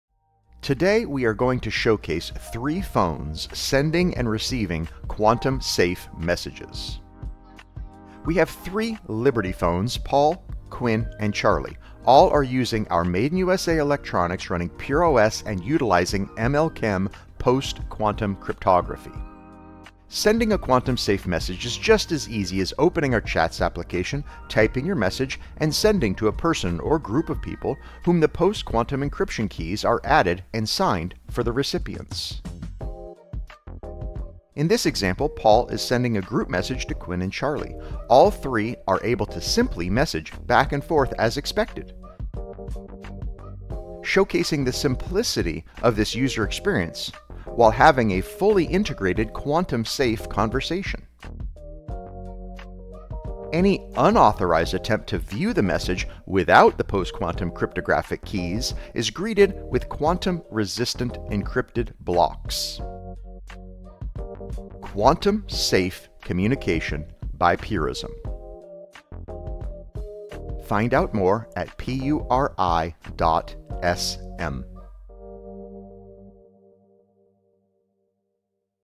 Quantum Safe Communication with Purism
Post Quantum Cryptography (PQC) is a critical advancement in securing data and communication, the threat of compromise is even greater than normal due to the fact that anything stored with pre-quantum resistant cryptography will be immediately available to view as soon as the qubits of quantum computing increase, which is a matter of when not if.